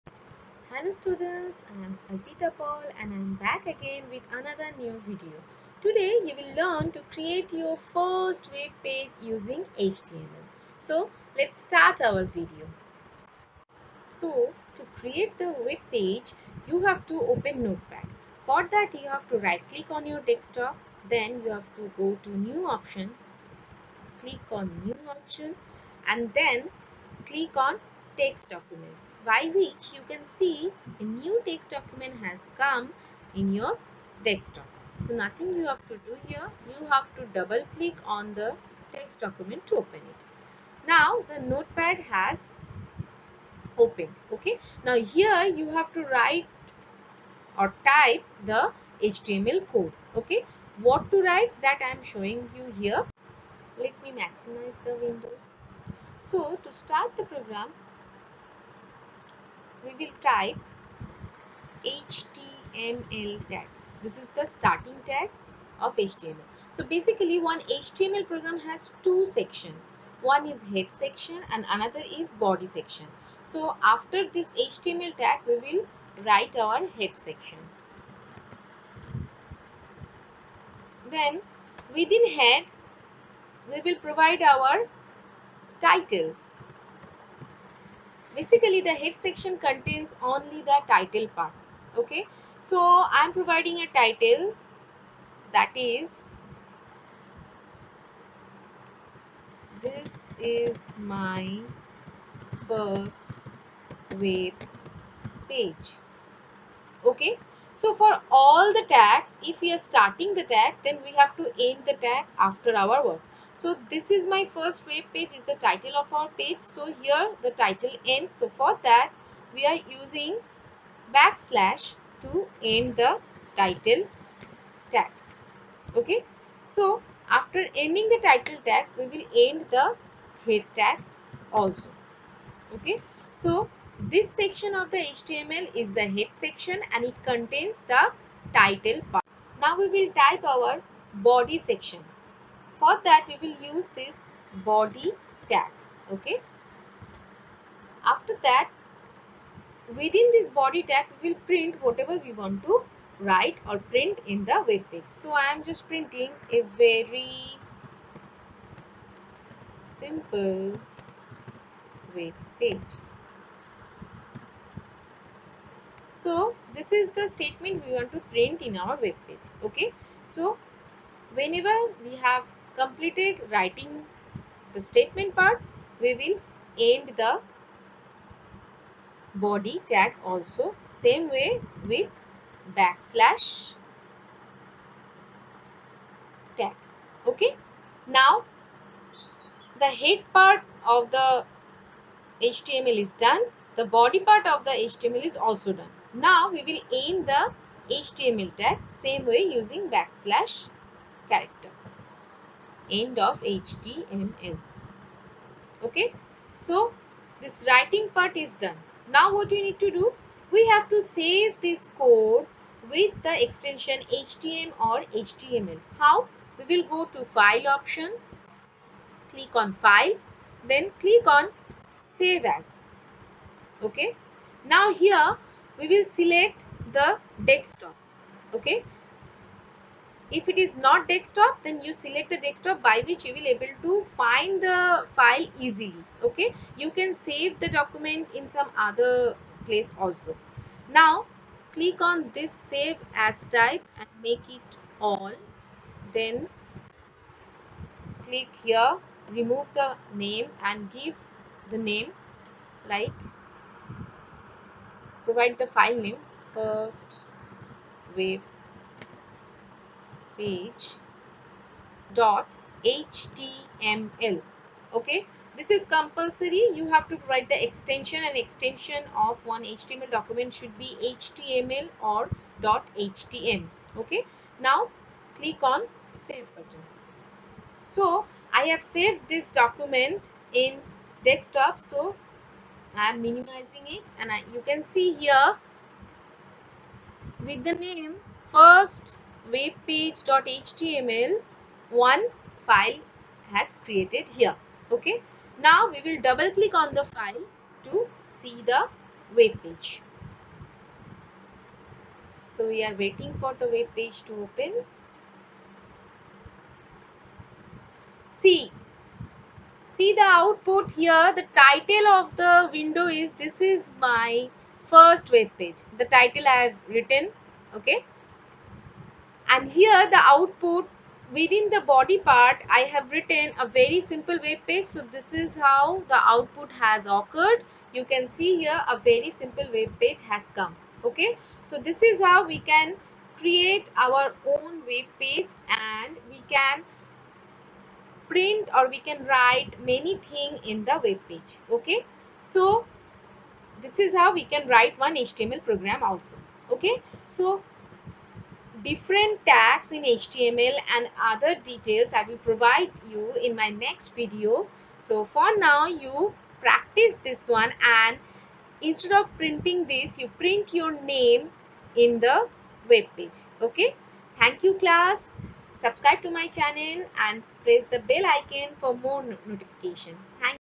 create your first webpage easily using HTML without any internet connection || HTML tutorial
Here you will learn how to create webpage using HTML in few simple steps.

Reference video links of related topics are given below.

Hope this will help.